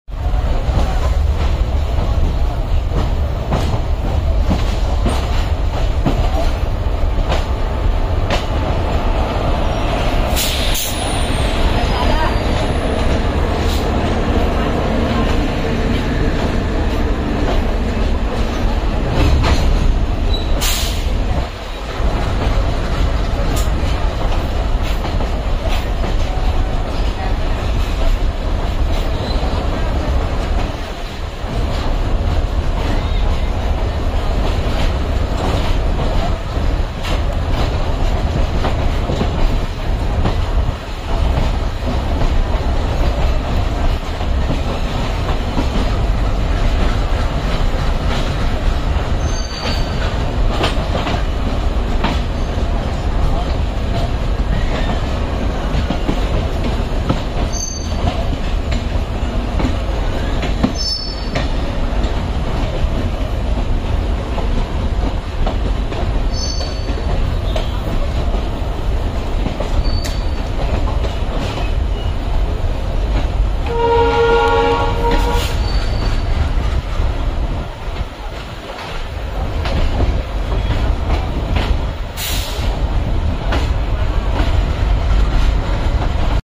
22732 : Mumbai - Hyderabad SF Exp Departing Pune JN With BSL WAP4 , Indian Railways
22732: Mumbai CSMT - Hyderabad Deccan Nampally Superfast Express Departing Pune Junction Behind Bhusaval WAP4 .